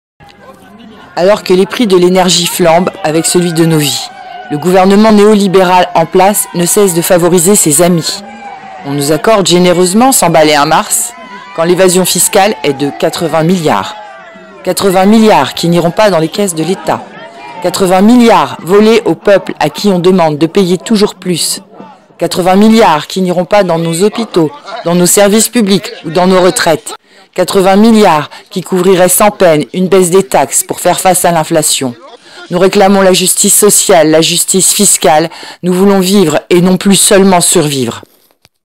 Justice sociale - Cortège des colères - convergence antipass#15
Cortège des colères - convergence antipass#15 - 23 octobre 2021
Bande sonore lors de la distribution des 80 milliards de l'évasion fiscale.

Alors que les prix de l'énergie flambe avec celui de nos vies.
Le gouvernement néo-libéral en place ne cesse de favoriser ses amis.

On nous accorde généreusement 100 balles et un mars.
Quand l'évasion fiscale est de 80 milliards.

80 milliards qui n'iront pas dans les caisses de l’État.
80 milliards volé au peuple à qui on demande de payer toujours plus.
80 milliards qui n'iront pas dans nos hôpitaux, dans nos services publics ou dans nos retraites.
80 milliards qui couvriraient sans peine une baisse des taxes pour faire face à l'inflation.

Justice sociale, justice fiscale.
Nous voulons vivre et plus seulement survivre.